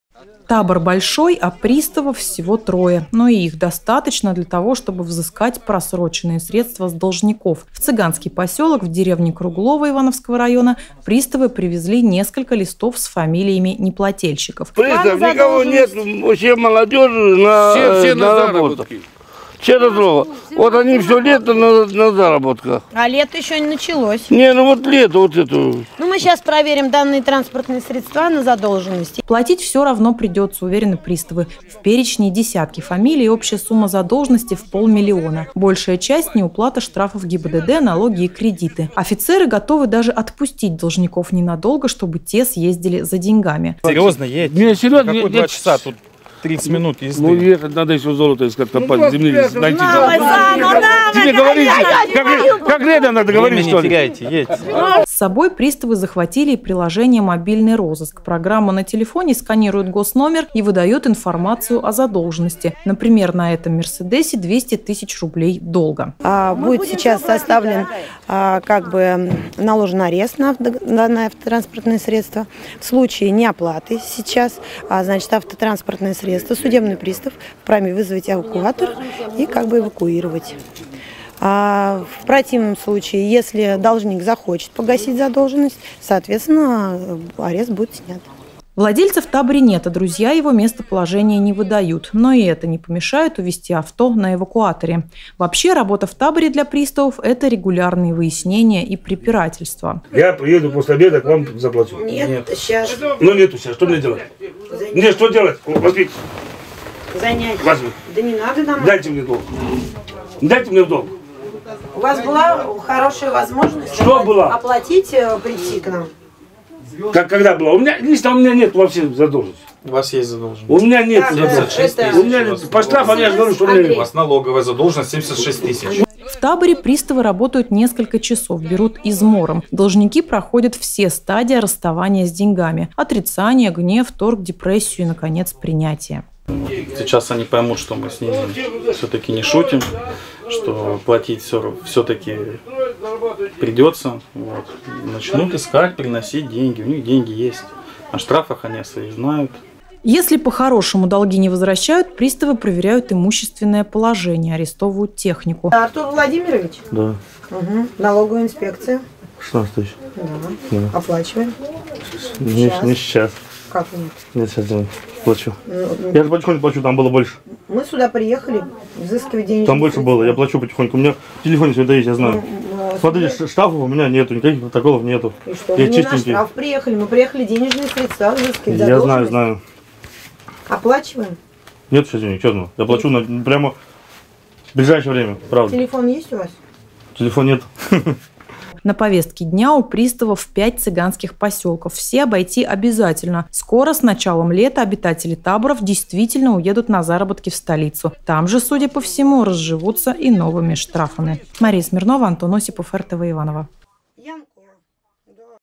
5 стадий расставаний с деньгами
Судебные приставы посетили табор в деревне Круглово Ивановского района и обнаружили задолженности на полмиллиона рублей.  По-хорошему отдавать деньги цыгане не захотели.